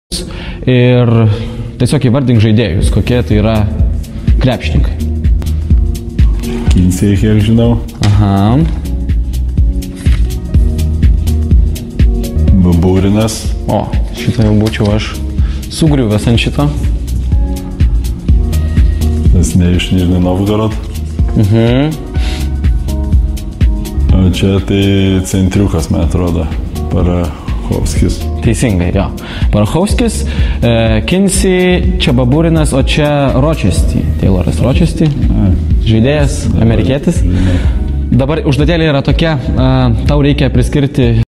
Arturas Milaknis doesn't recognize Taylor Rochestie
From Zalgiris TV (channel Zalgiris's guard Anturas Milaknis doesn't recognize Nizhny Novgorod star Taylor Rochestie by his photo - before the two teams meet in Euroleague for the first time ever.